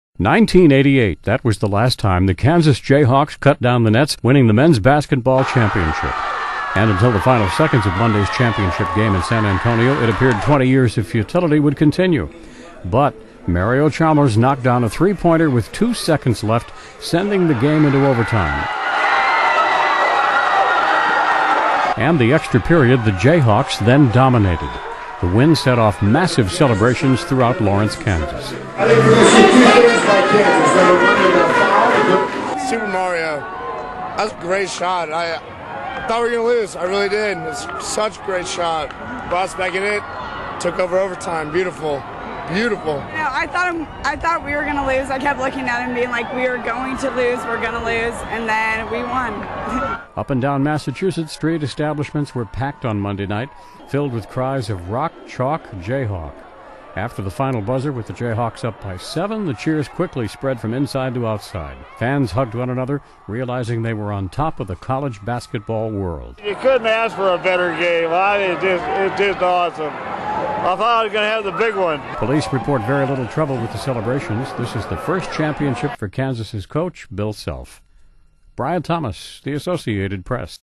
Massive Celebrations Following Overtime Win
The Kansas Jayhawks are the 2008 men's basketball champions.  The Jawhawks beat the Memphis Tigers in overtime, setting off huge celebrations throughout Lawrence.